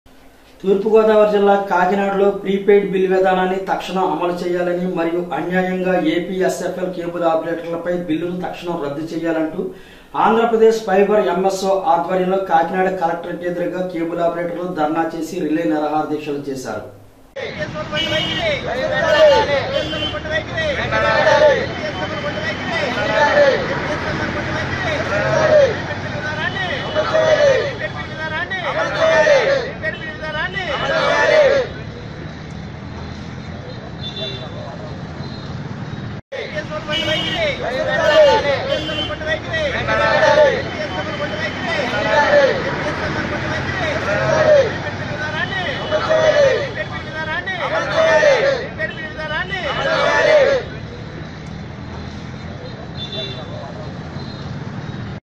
తూర్పుగోదావరి జిల్లా కలెక్టరేట్ ఎదుట అన్యాయంగా ఏ.పి.ఏస్.ఏప్.ఏల్.కేబుల్ ఆపరేటర్ల పై బిల్లును తక్షణం
తూర్పుగోదావరి జిల్లా కలెక్టరేట్ ఎదుట అన్యాయంగా ఏ.పి.ఏస్.ఏప్.ఏల్.కేబుల్ ఆపరేటర్ల పై బిల్లును తక్షణం రద్దు చేయ్యాలి